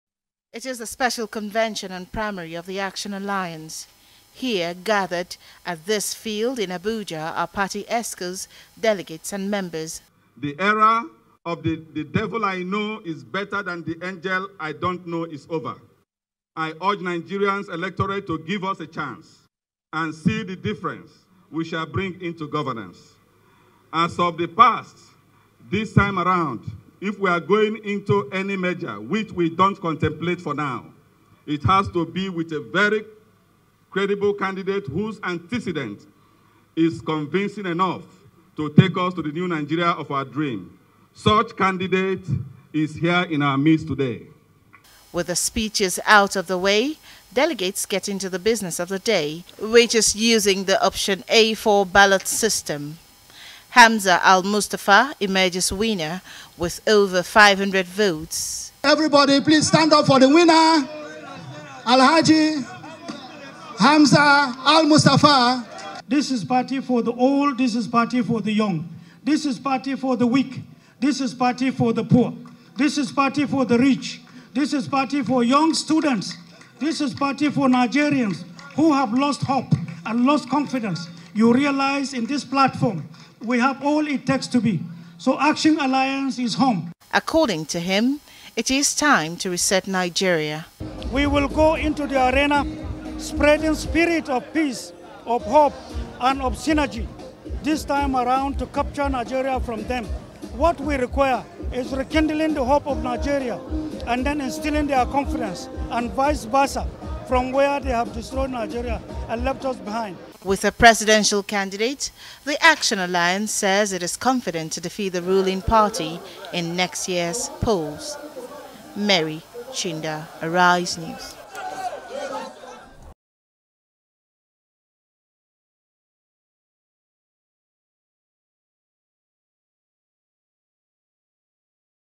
AL-MUSTAPHA EMERGES ACTION ALLIANCE PRESIDENTIAL CANDIDATE - ARISE NEWS REPORT
Hamza Al -Mustapha, former Chief Security Officer to the late military ruler Sani Abacha has emerged the presidential flag bearer of the Action Alliance for the 2023 presidential elections. Arise Correspondent, Mary Chinda, monitored the party's primary and reports.